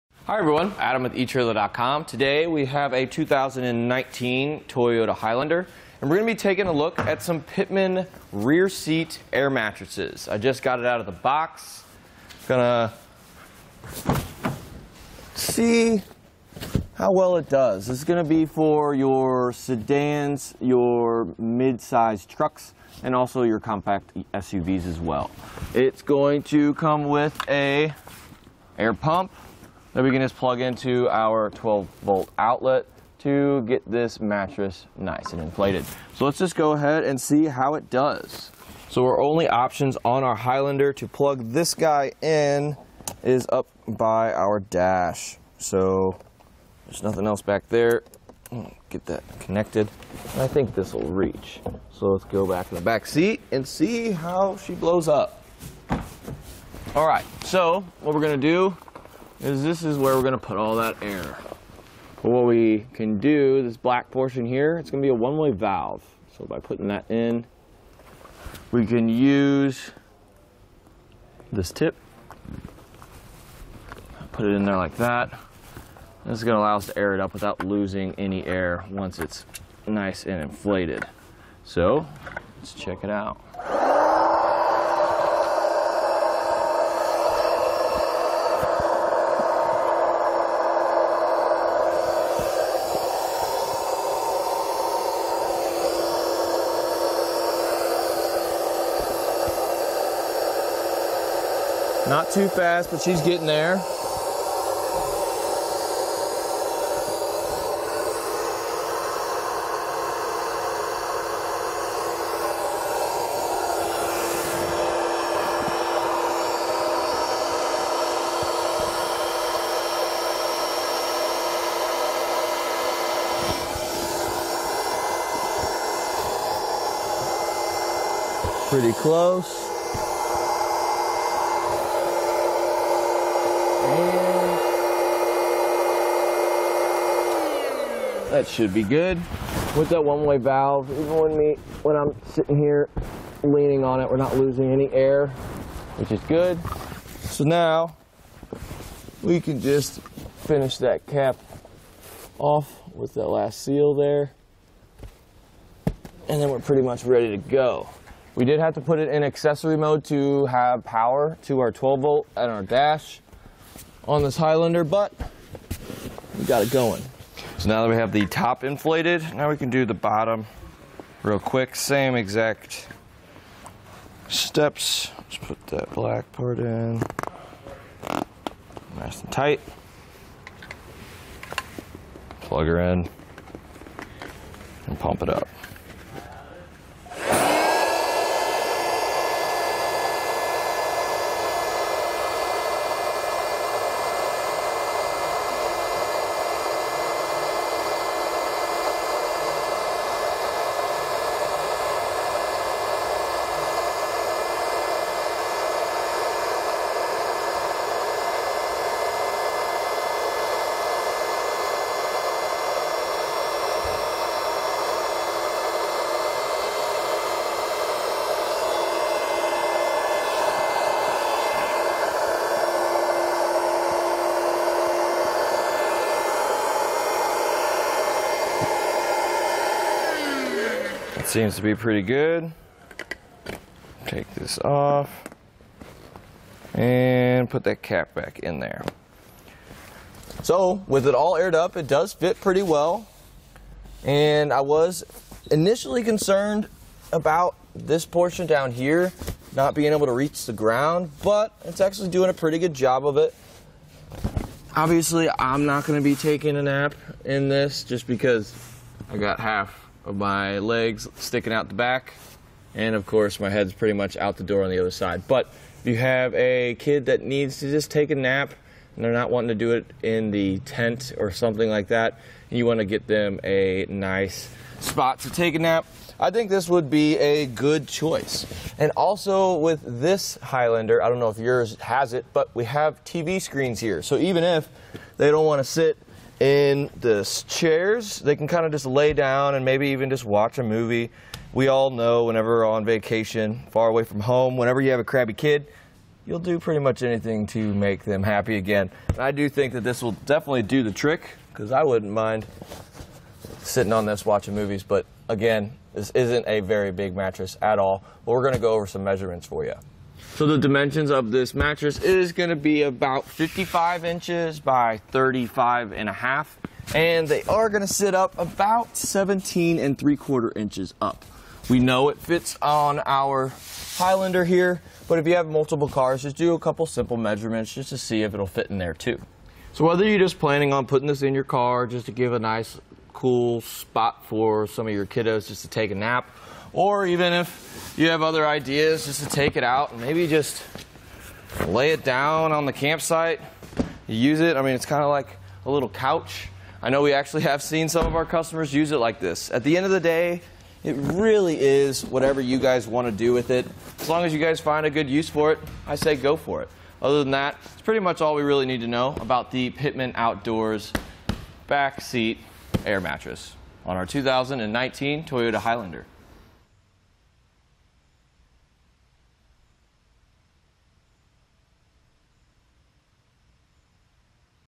etrailer | Set up: AirBedz Rear Seat Air Mattress w Portable 12V Pump on a 2019 Toyota Highlander
Adam:                                                  Hi, everyone. Adam with etrailer.com. Today we have a 2019 Toyota Highlander, and we're going to be taking a look at some Pittman rear-seat air mattresses. I just got it out of the box. Going to see how well it does. This is going to be for your sedans, your mid-size trucks and also your compact SUVs as well.

It's going to come with an air pump that we can just plug into our 12-volt outlet to get this mattress nice and inflated.So let's just go ahead and see how it does. So our only options on our Highlander to plug this guy in, is up by our dash, so there's nothing else back there. Get that connected. I think this will reach. So let's go back to the back seat and see how she blows up.All right, so what we're going to do is, this is where we're going to put all that air.

Or we can do this black portion here. It's going to be a one-way valve. So by putting that in, we can use this tip. Put it in there like that. This is going to allow us to air it up without losing any air once it's nice and inflated.

So let's check it out.Not too fast but she's getting there.Pretty close.That should be good. With that one-way valve, even when I'm sitting here leaning on it, we're not losing any air, which is good. So now we can just finish that cap off with the last seal there and then we're pretty much ready to go.We did have to put it in accessory mode to have power to our 12-volt at our dash on this Highlander. But we got it going.So now that we have the top inflated, now we can do the bottom real quick. Same exact steps.

Just put that black part in nice and tight. Plug her in and pump it up.It seems to be pretty good. Take this off and put that cap back in there.So with it all aired up, it does fit pretty well. And I was initially concerned about this portion down here, not being able to reach the ground, but it's actually doing a pretty good job of it. Obviously I'm not going to be taking a nap in this just because I've got half of my legs sticking out the back. And of course my head's pretty much out the door on the other side. But you have a kid that needs to just take a nap and they're not wanting to do it in the tent or something like that and you want to get them a nice spot to take a nap, I think this would be a good choice.And also with this Highlander, I don't know if yours has it, but we have TV screens here, so even if they don't want to sit in this chairs, they can kind of just lay down and maybe even just watch a movie. We all know whenever on vacation, far away from home, whenever you have a crabby kid, you'll do pretty much anything to make them happy again. And I do think that this will definitely do the trick, because I wouldn't mind sitting on this watching movies. But again, this isn't a very big mattress at all, but we're going to go over some measurements for you.So the dimensions of this mattress is going to be about 55 inches by 35 and a half and they are going to sit up about 17 and three quarter inches up. We know it fits on our Highlander here, but if you have multiple cars, just do a couple of simple measurements just to see if it'll fit in there too.So whether you're just planning on putting this in your car, just to give a nice cool spot for some of your kiddos just to take a nap or even if you have other ideas just to take it out and maybe just lay it down on the campsite. You use it. I mean it's kind of like a little couch. I know we actually have seen some of our customers use it like this. At the end of the day, it really is whatever you guys want to do with it, as long as you guys find a good use for it, I say go for it. Other than that, it's pretty much all we really need to know about the Pittman Outdoors back-seat air mattress on our 2019 Toyota Highlander.